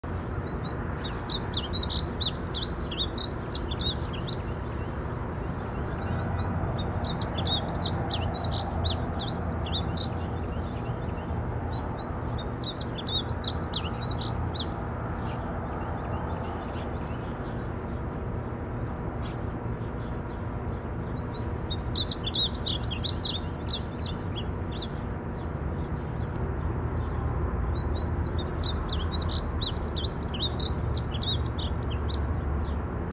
Painted bunting2
Splotches of color and a rolling song, painted buntings are always a delight to see.